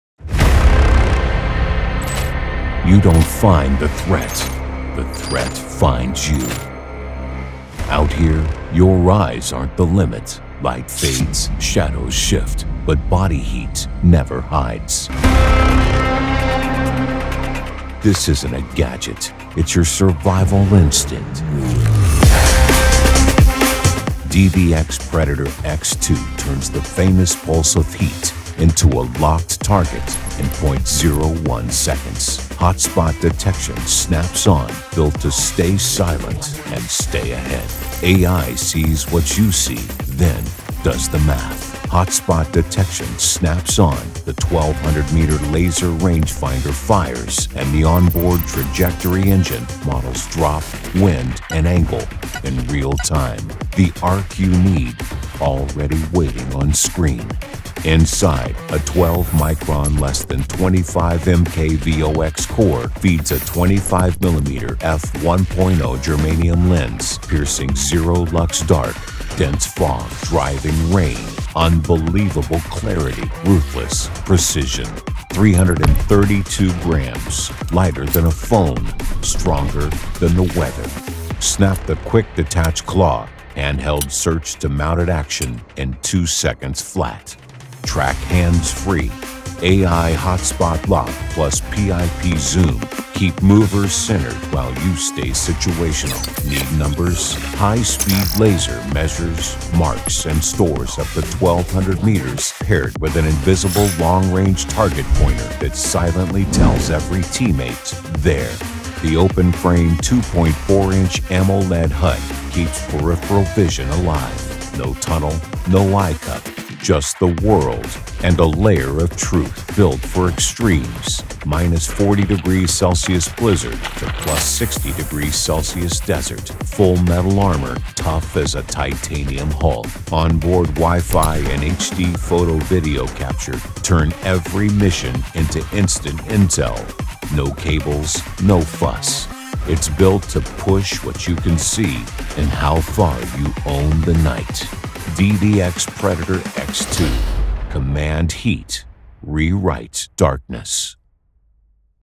DVX Predator X2
Military-Class AI Thermal Scope for Rifles and Handheld Use. DVX Predator X2 is now live on Indiegogo, check